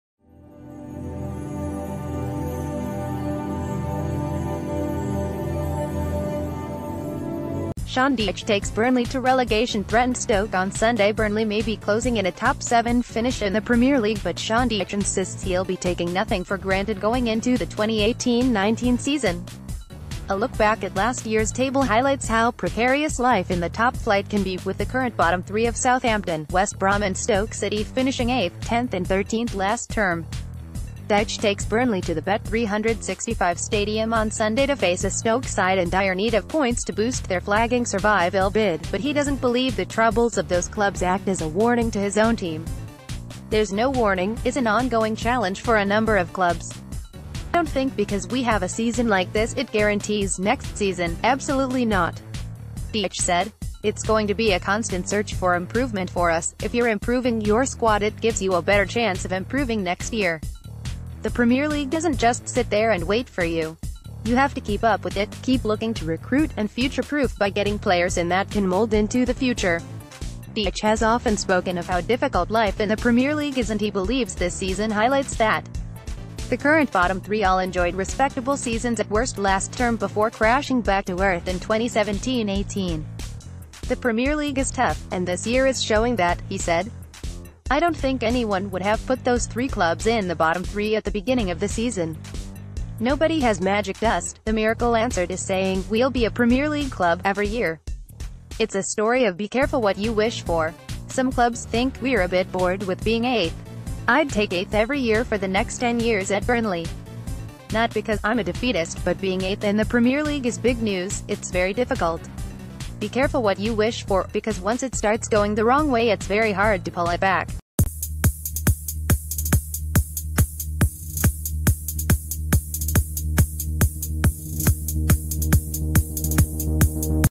Breaking News | Sean Dyche: Relegation candidates show how tough top flight is
Sean Dyche takes Burnley to relegation threatened Stoke on Sunday BURNLEY may be closing in a top-seven finish in the Premier League but Sean Dyche insists he’ll be taking nothing for granted going into the 2018/19 season. A look back at last year’s table highlights how precarious life in the top flight can be, with the current bottom three of Southampton, West Brom and Stoke City finishing eighth, 10th and 13th last term. Dyche takes Burnley to the Bet365 Stadium on Sunday to face a Stoke side ...